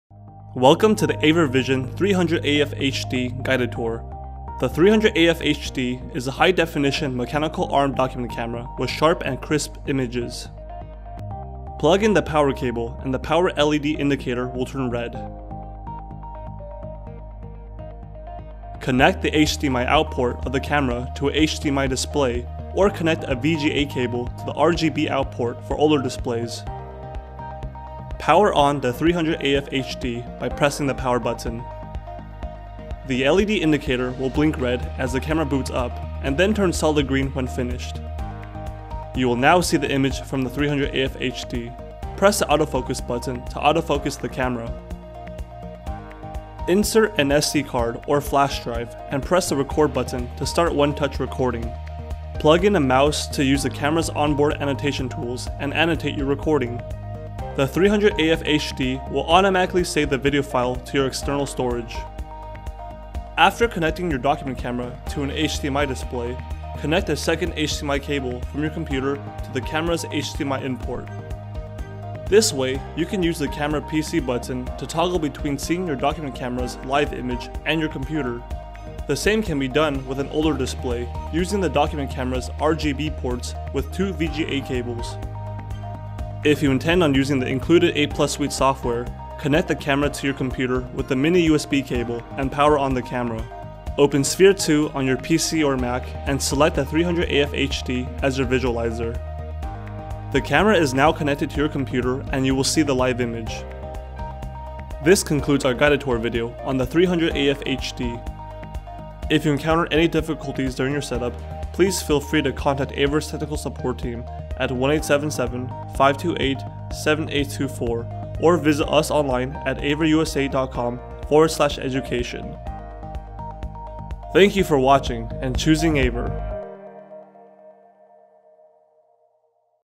AVer 300AFHD Document Camera Guided Tour
Got HDMI? HDMI Output, Auto Focus and 30fps display make the smallest details sharp and crisp - with AVer's 300AFHD Document Camera. This is the legacy mechanical arm document camera that established AVer's high quality reputation and transformed the learning environment for students and teachers nationwide.

00:11 - Connecting to HDMI/RGB
00:39 - One Touch Recording/Annotation
00:57 - HDMI/RGB Passthrough
01:20 - Connecting via USB